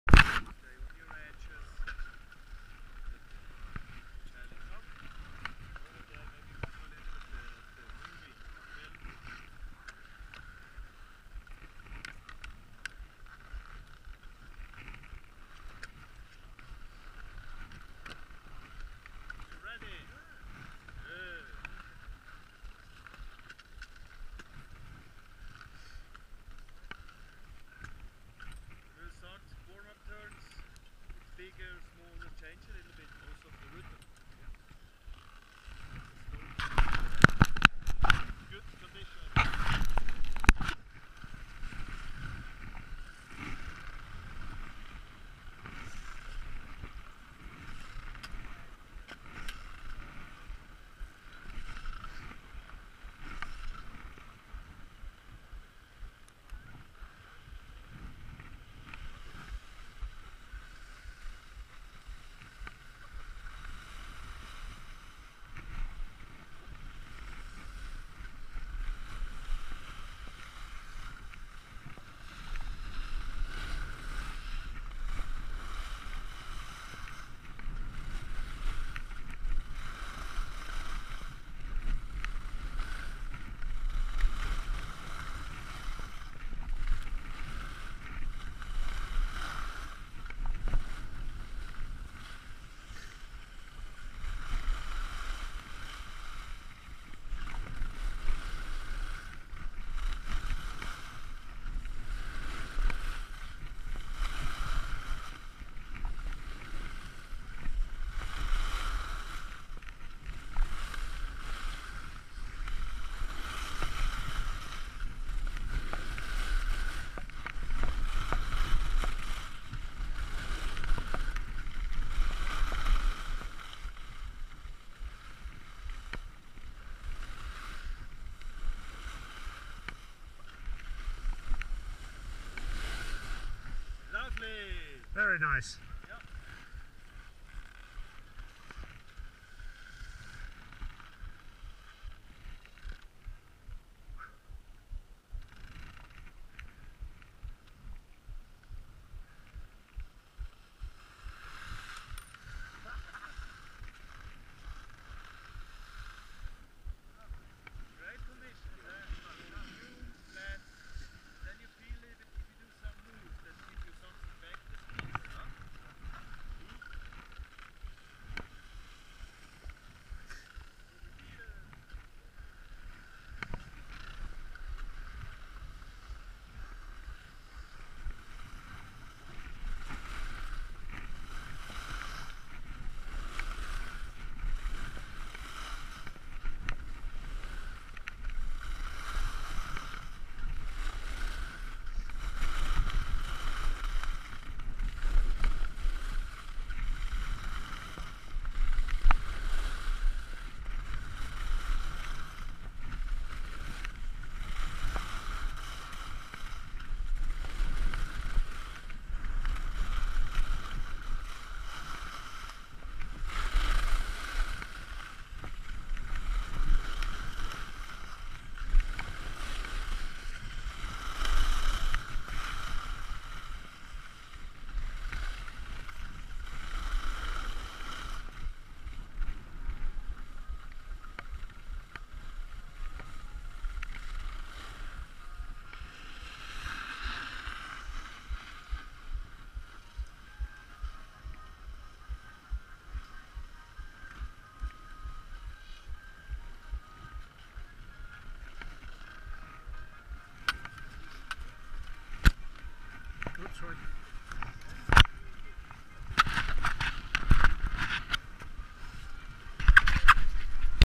Saas Fee - Skiing Langfluh
Skiing Langfluh, level 5 with instructor Ingamar